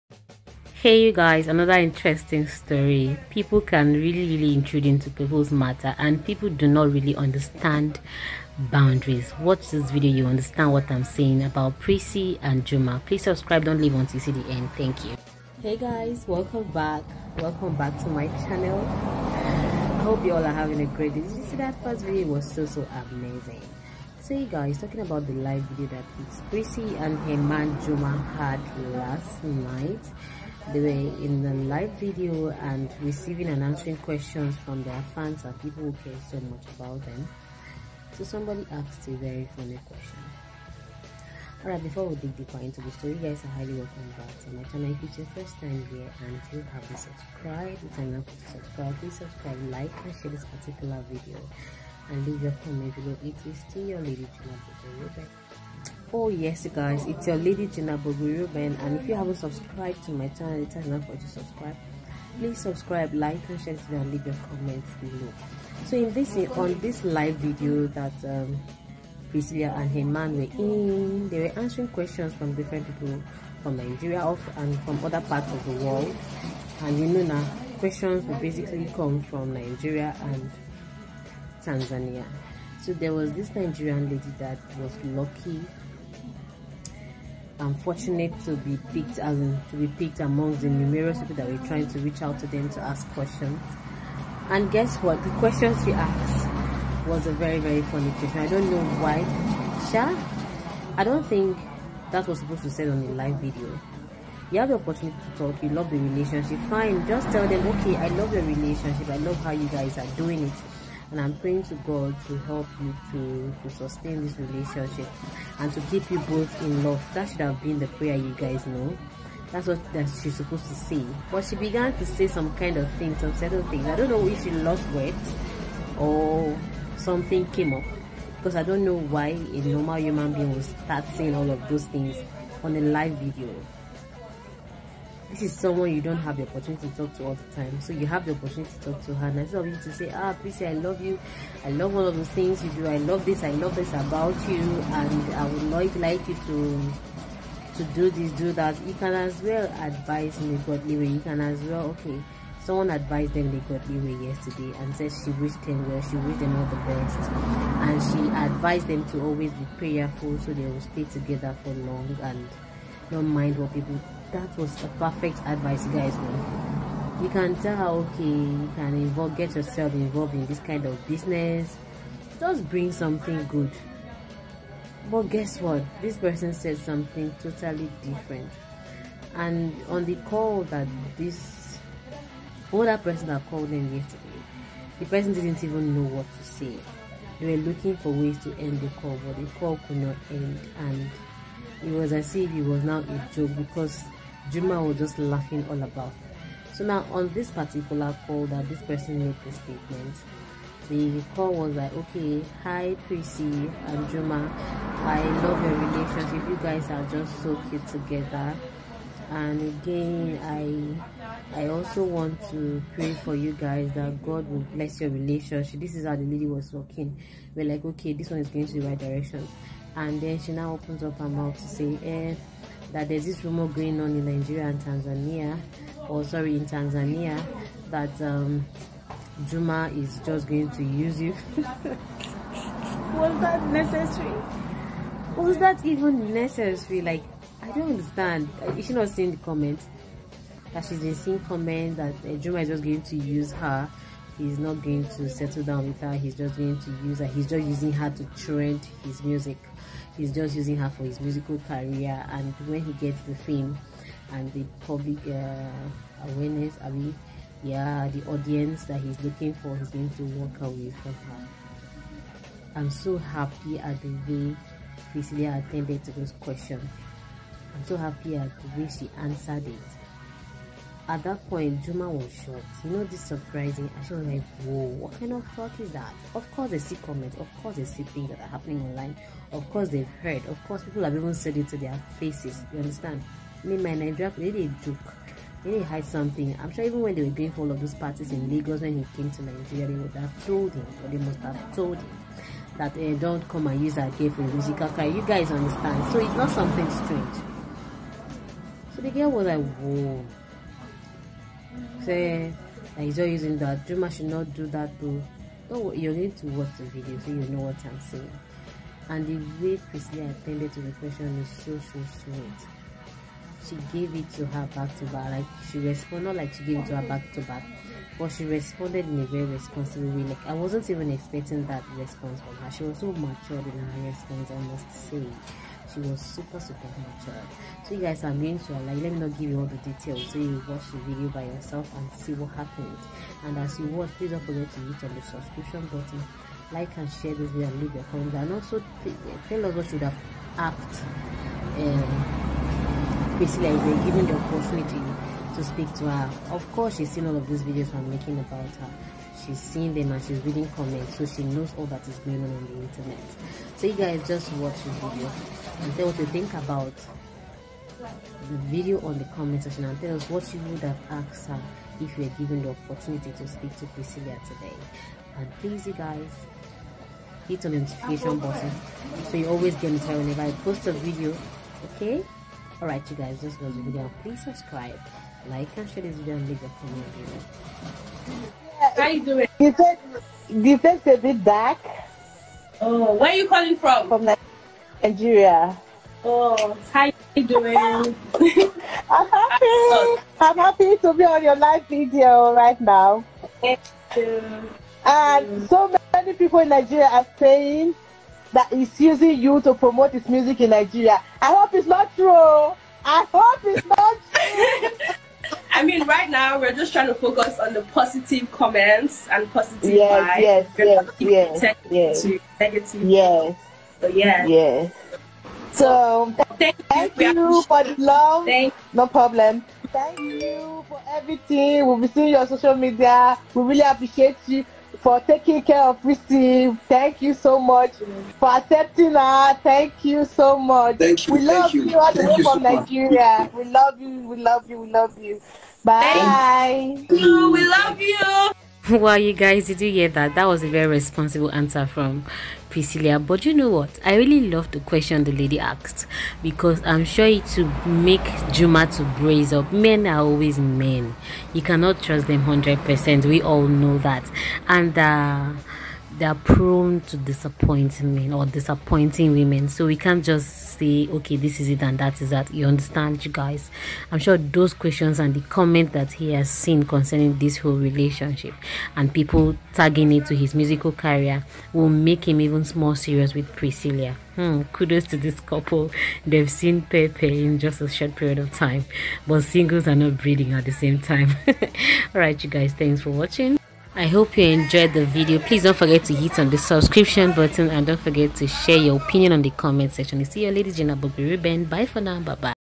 Iyabo Ojo's daughter Priscy and husband question and answer section hear what his ex asked them
Love is sweet when you do it with the right person